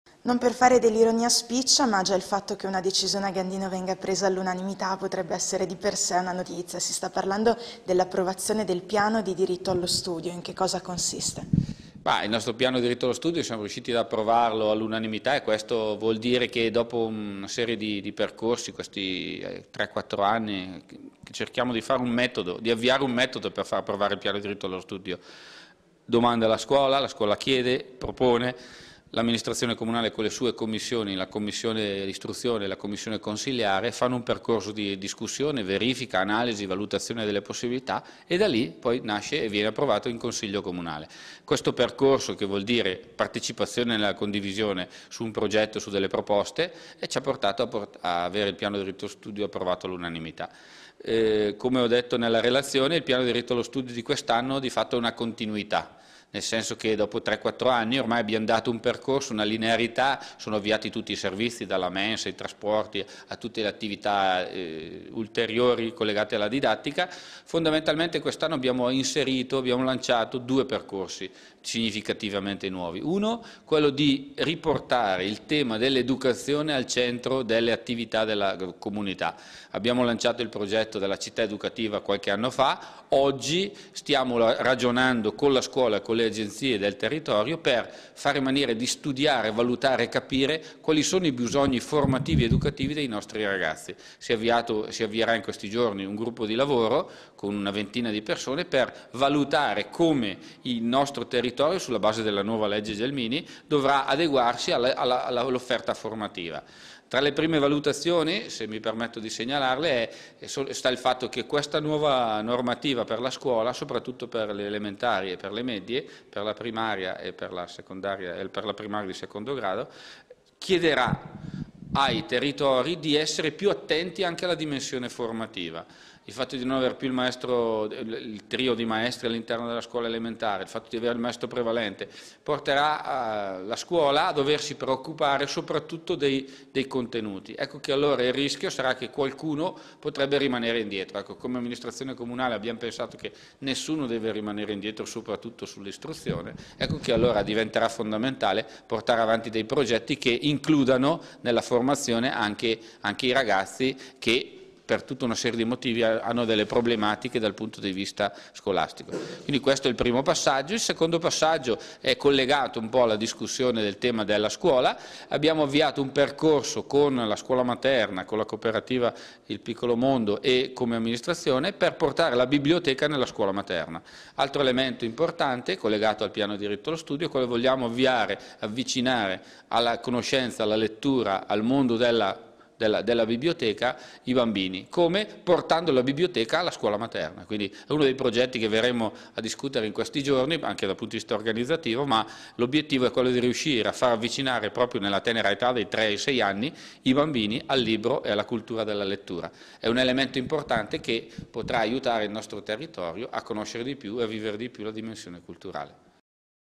Gandino_unanimità_per_il_diritto_allo_studio_Antenna_2_TV_201010.avi
A Gandino il Consiglio comunale ha approvato all'unanimità il Piano per il diritto allo studio.